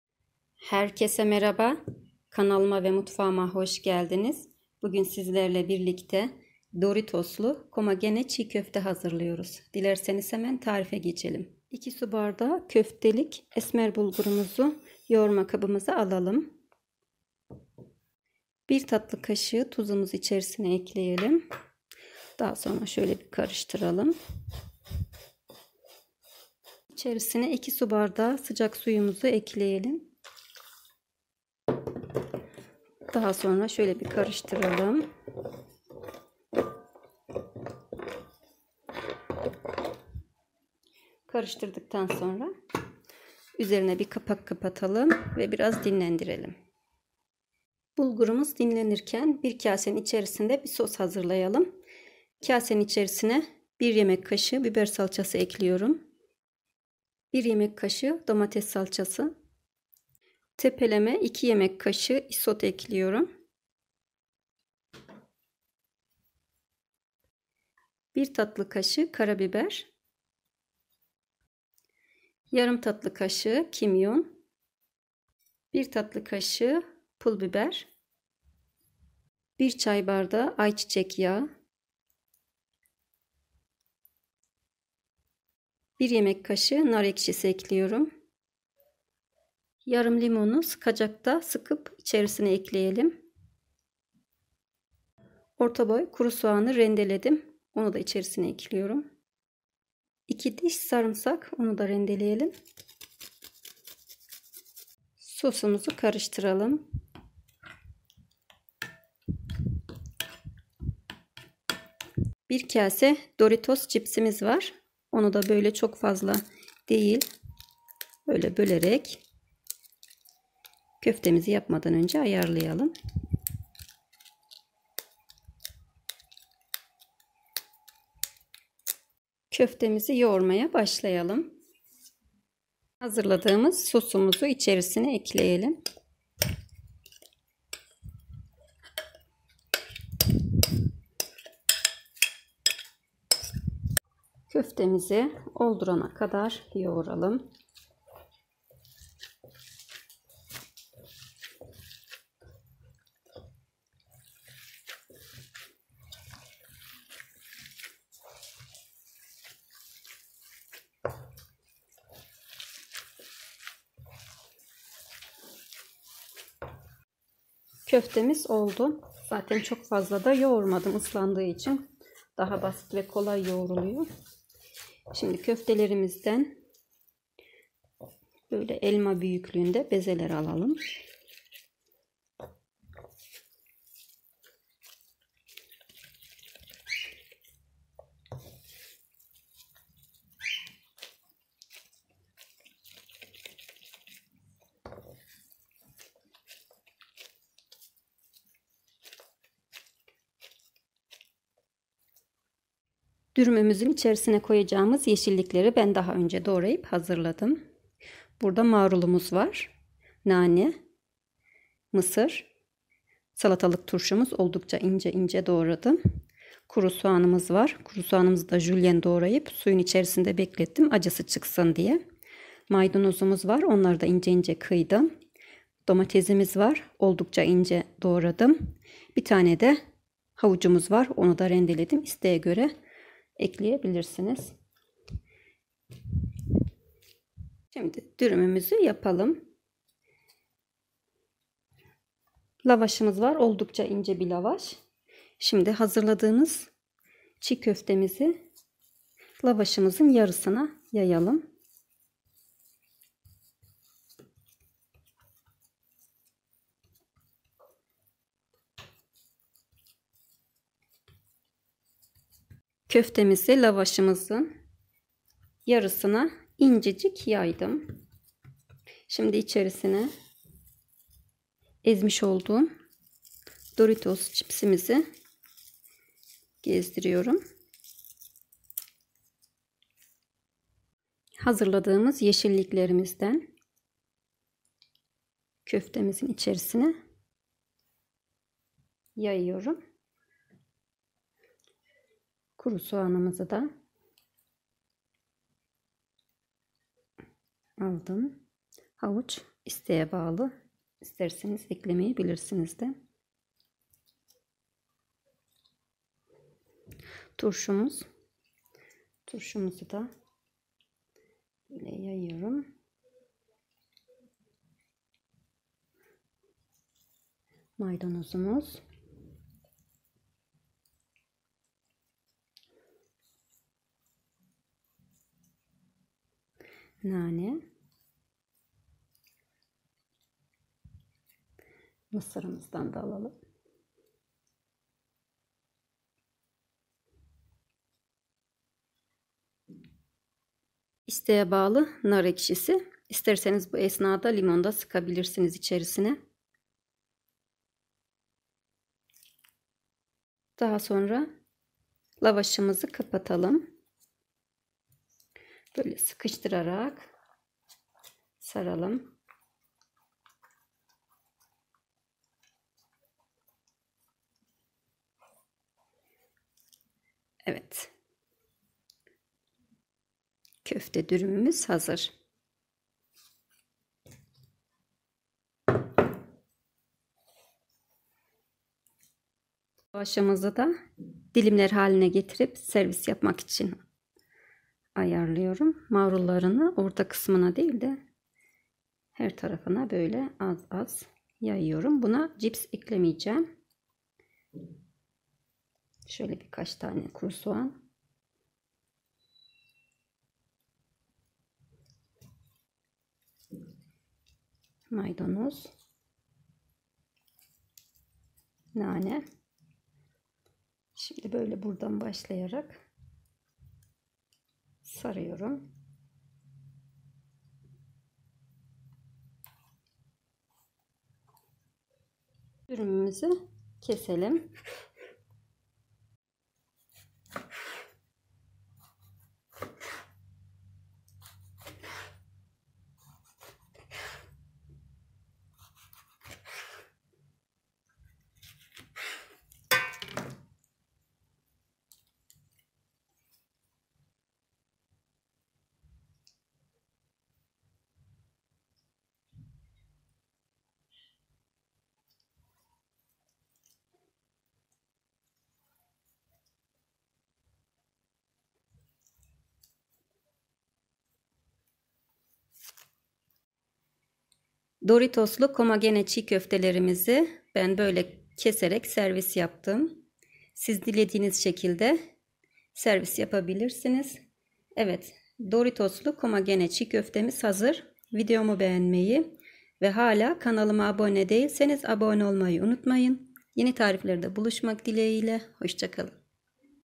DORİTOSLU KOMAGENE ÇİĞ KÖFTE-KOMAGENE ÇİĞ KÖFTE TARİFİ-ÇİĞ KÖFTE DÜRÜM NASIL YAPILIR-ETSİZ ÇİĞ KÖFTE
Merhabalar 🙋 bugün sizlere herkesin severek yediği KOMAGENE ÇİĞ KÖFTE TARİFİ verdim, İster LAVAŞA serip ÇİĞ KÖFTE DÜRÜM yapın. isterseniz anında uğraşmadan tüketin o kadar lezzetli, o kadar iddialı 🙈🥳

ÇİĞ KÖFTE DÜRÜM NASIL YAPILIR,
ÇİĞ KÖFTE DÜRÜM TARİFİ,
KOMAGENE ÇİĞ KÖFTE,
KOMAGENE ÇİĞ KÖFTE EVDE NASIL YAPILIR

Köftesi için malzemeler
2 su bardağı köftelik esmer bulgur
2 su bardağı sıcak su
1 tatlı kaşığı tuz

Çiğ köfte sosu için malzemeler
1 yemek kaşığı domates salçası
1 yemek kaşığı biber salçası
2 tepeleme yemek kaşığı isot
1 çay bardağı ayçiçek yağı
1 tatlı kaşığı karabiber
yarım tatlı kaşığı kimyon
1 tatlı kaşığı pul biber
1 yemek kaşığı nar ekşisi
2 diş sarımsak
1 orta boy kurusoğan

Yeşillik malzemeleri
Doritos cips
marul
nane
maydanoz
kurusoğan
domates
mısır
salatalık turşusu
havuç

İnce lavaş